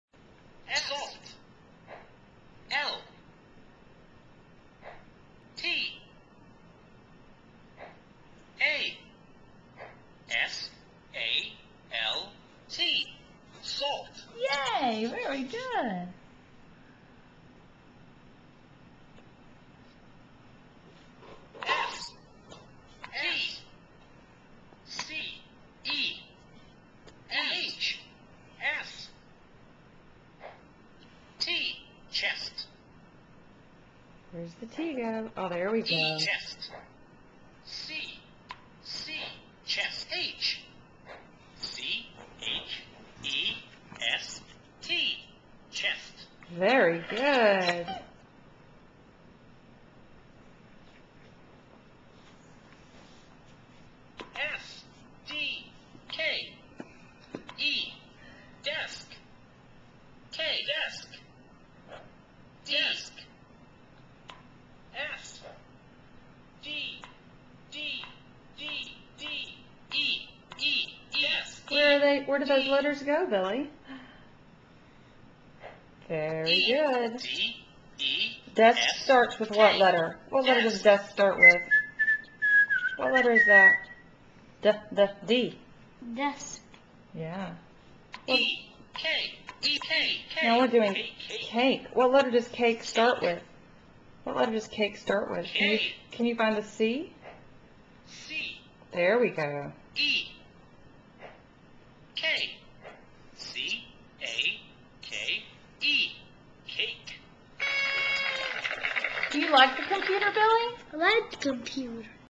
Billy gets an iPad pt 2
Our autistic 4-year-old, Billy is practicing with a First Words app on his new iPad. This is the 2nd time he's used it.